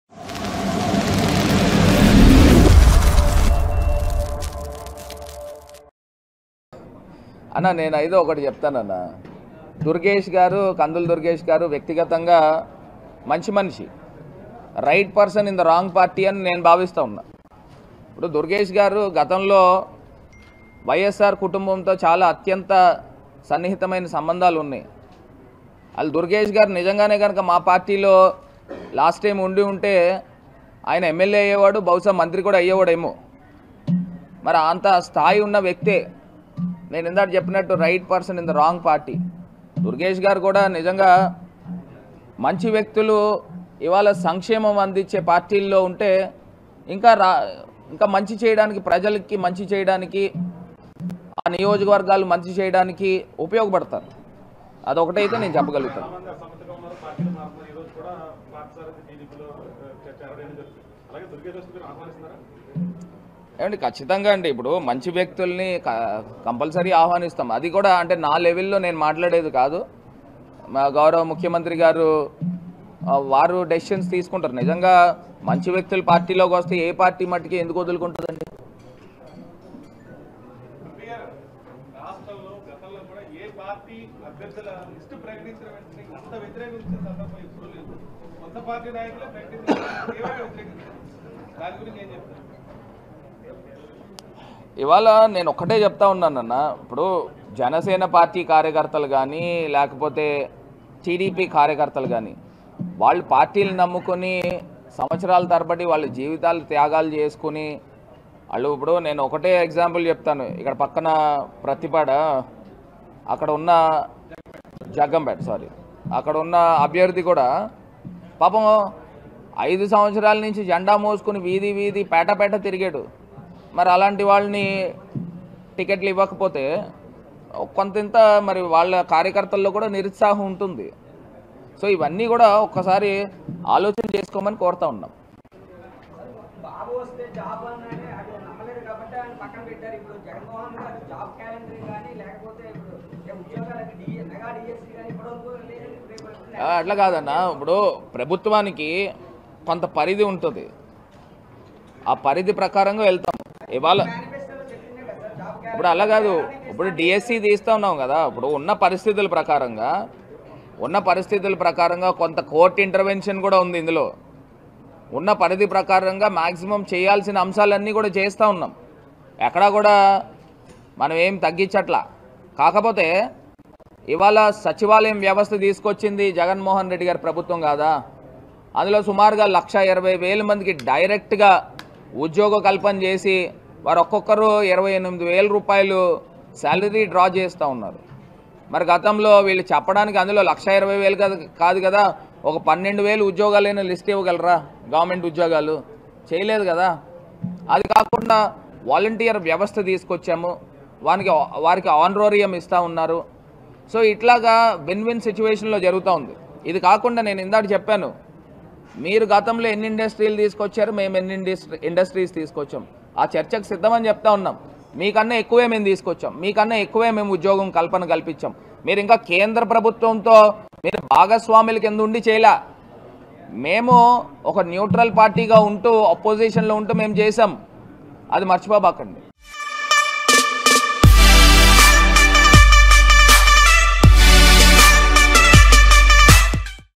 MP Bharath about Kandula Durgesh Joining In YCP | AP Elections 2024 | Rajahmundry News | Socialpost
YCP MP Bharath about Kandula Durgesh Joining In YCP | AP Elections 2024 | Rajahmundry Politics | YCP Vs Janasena | Socialpost Politics